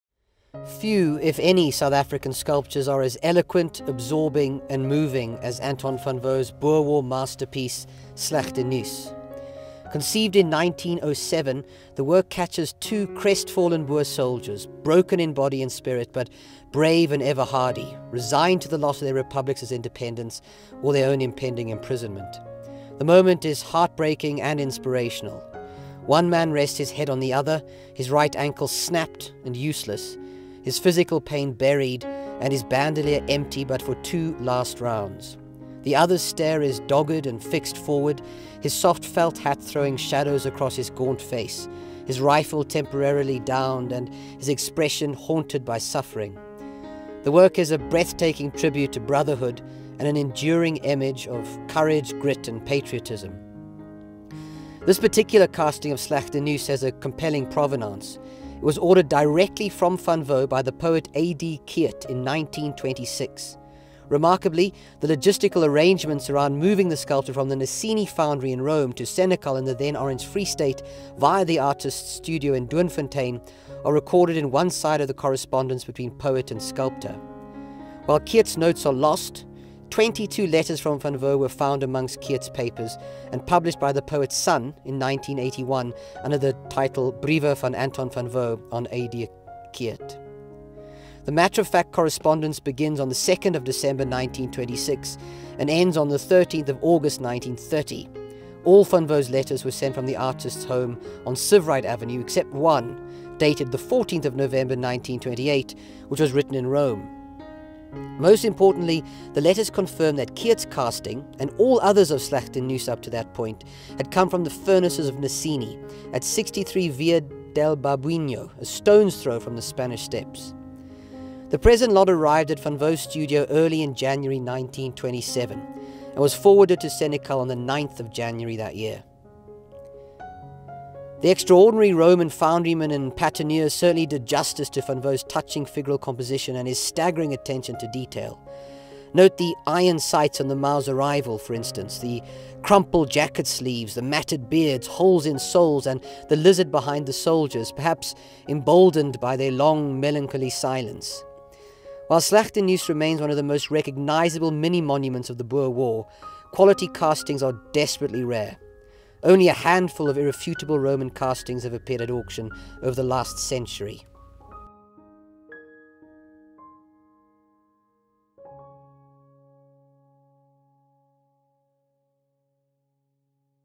Slegte Nuus by Anton van Wouw (1907)
Strauss & Co Senior Art Specialist Dr Alastair Meredith discusses an important and unique work by South African sculptor Anton van Wouw, titled 'Slegte Nuus', from the April 2021 Live Virtual Auction.

Going under the hammer on Tuesday 13 April 2021.
View the item:
0:12 - Van Wouw
Auction details:
11-13 April 2021
19th Century, Modern, Post-War and Contemporary Art, Decorative Arts, Jewellery and Wine
Brickfield Canvas, 35 Brickfield Road, Woodstock, Cape Town